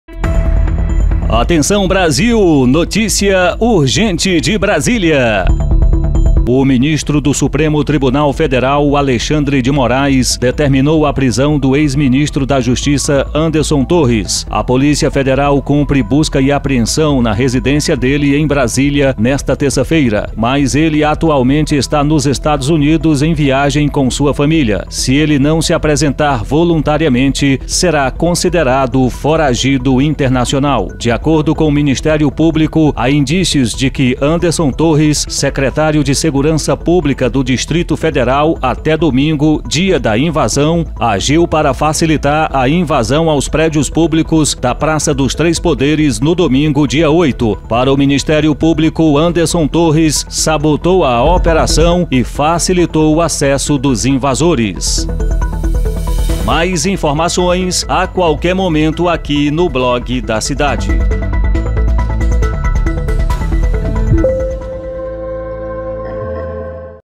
Moraes Manda prender Anderson Torres Polícia Federal cumpre mandato na casa de
Moraes Manda prender  Anderson Torres  Polícia Federal cumpre mandato na casa de Anderson Torres
PF faz operação na casa de Anderson Torres